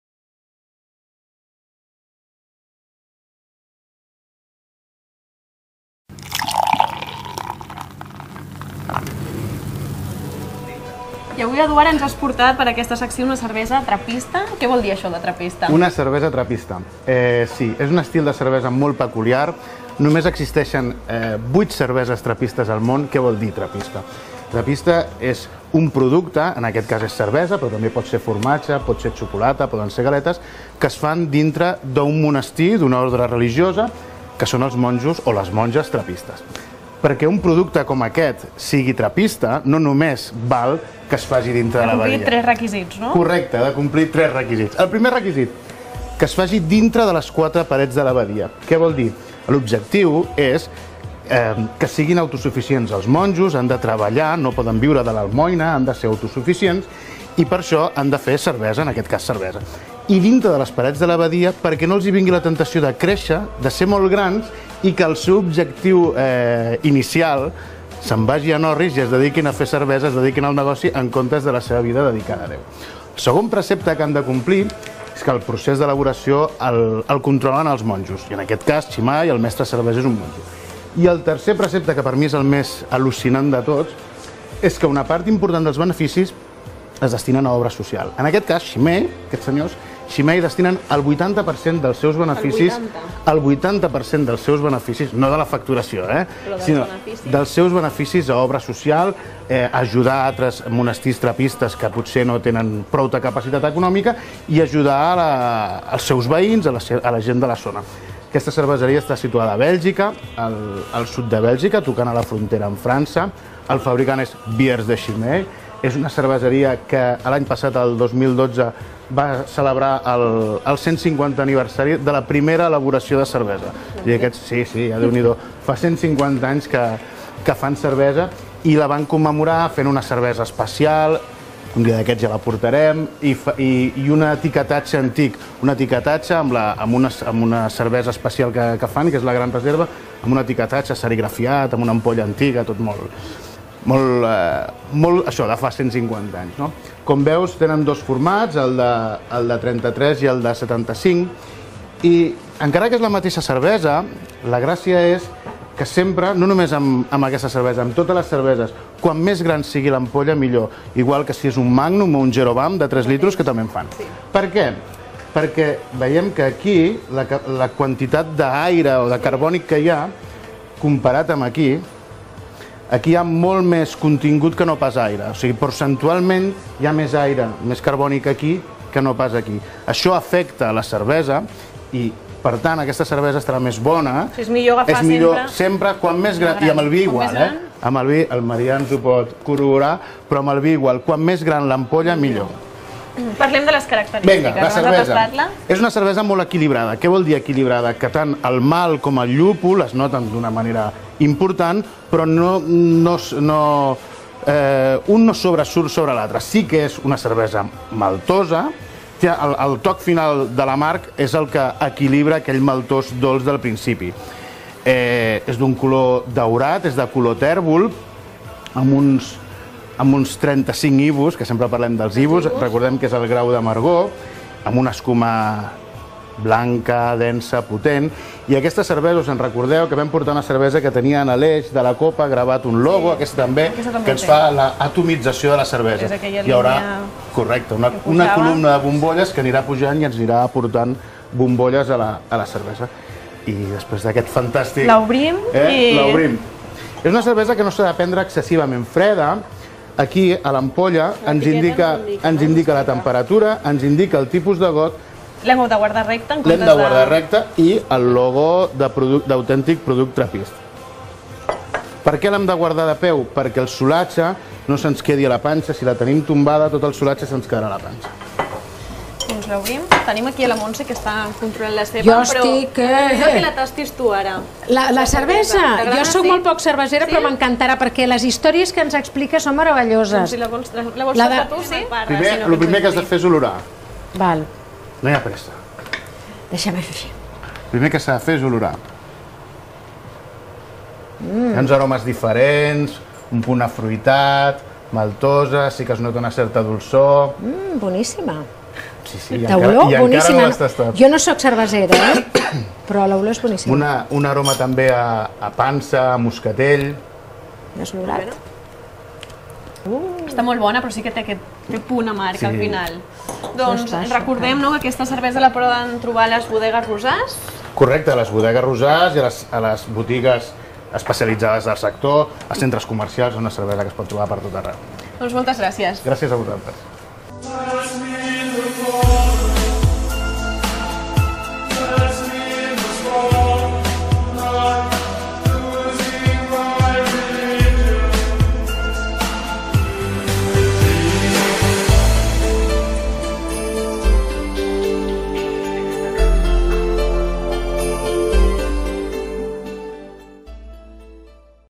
Mentre Cuinem, Cerveza Chimay Tripel
Eduard Bröll, de Crusat, nos presenta CHIMAY TRIPEL, cerveza belga de tipo Tripel Trapista (8%) elaborada por Chimay.